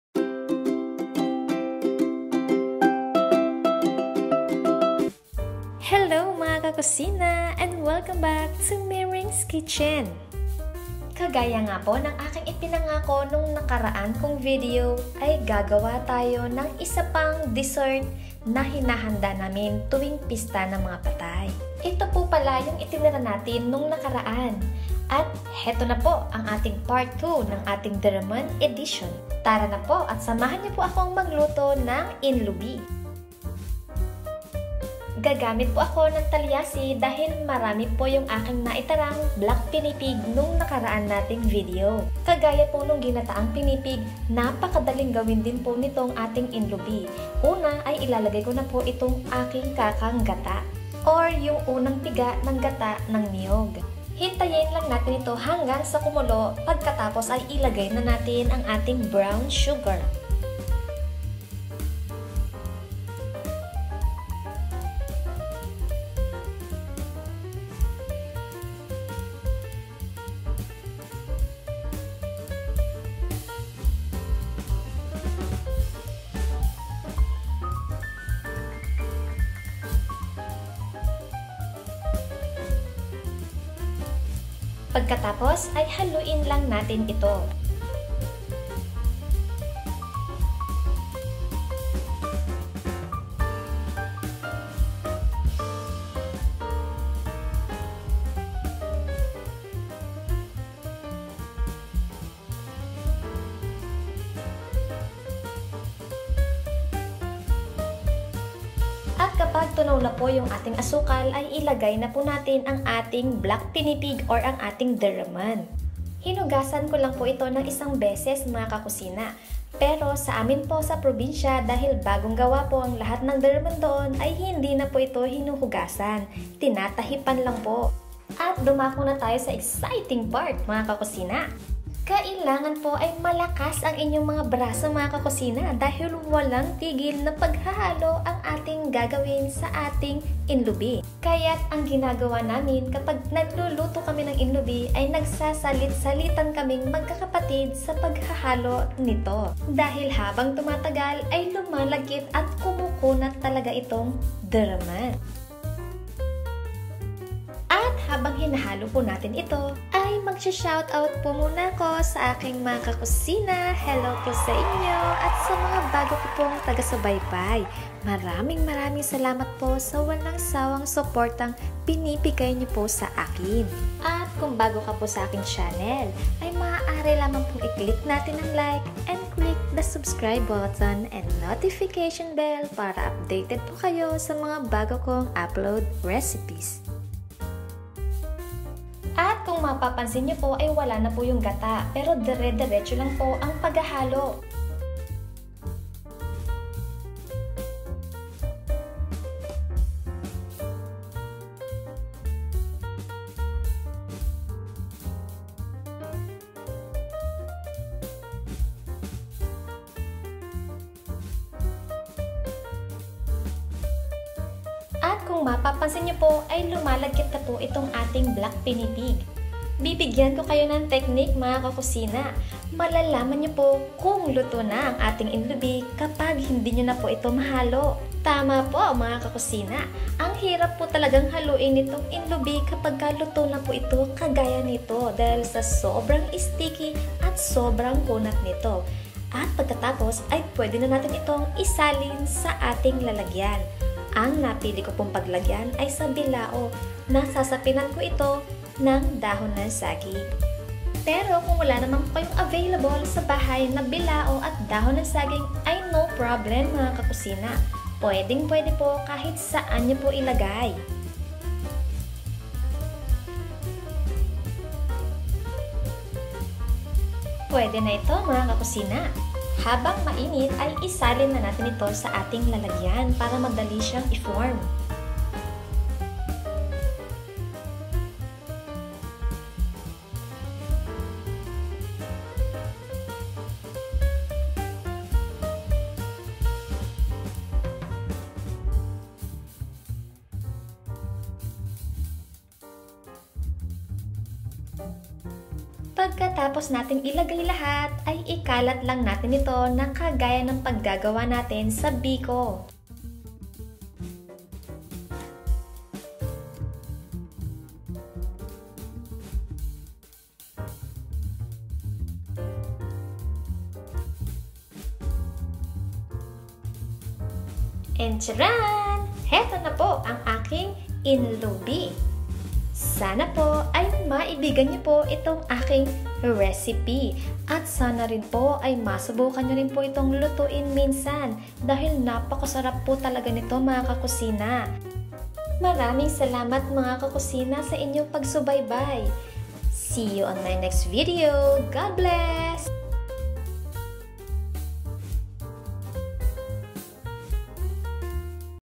MASARAP PA ITO SA BIKO!.. INLUBI (PART 2 Deremen edition Halloween Dessert ) PANGASINAN DELICACY
Sa Video na ito ay ipapakita ko sa inyo kung ano ang madalas naming ihain tuwing Pista ng mga Patay. Ito kasi ay inaani tuwing katapusan ng Oktubre hanggang unang linggo ng Nobyembre. Sana po ay panuorin ninyo mga ka kusina at maibigan ninyo itong dessert na ito.

Maraming salamat po and God Bless!..